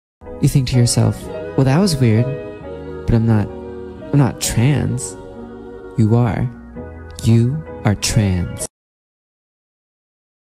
But I’m not.. I’m not trans… // Ft. Irl me & My OC // (yep I’m trans) ^^
I’m gonna use this to come out to my mother ;-; good luck meeeee